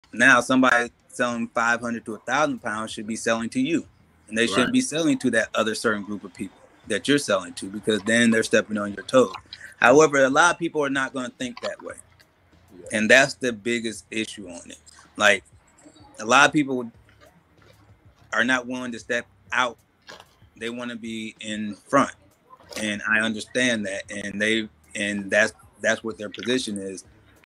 Mastering Niche Marketing: Strategies for High-Value Sales
In this insightful video, we delve into the critical aspects of niche marketing, specifically focusing on the dynamics of selling high-value items ranging from 500 to 1000 pounds. We unravel why understanding your audience is pivotal for avoiding competition and ensuring your sales strategy stands out. Whether you're a seasoned marketer or a budding entrepreneur, this video will equip you with the knowledge to target the right audience effectively, avoid common pitfalls, and excel in your niche. Join us as we explore the nuances of market segmentation, audience analysis, and strategic positioning to help you achieve your sales goals without stepping on your competitors' toes.

Chapter Markers:
(00:00) Introduction to High-Value Sales
(00:15) Understanding Your Target Audience
(00:30) The Importance of Niche Marketing
(00:45) Avoiding Competition and Overlap
(01:00) Strategies for Effective Targeting
(01:15) Conclusion: Standing Out in Your Niche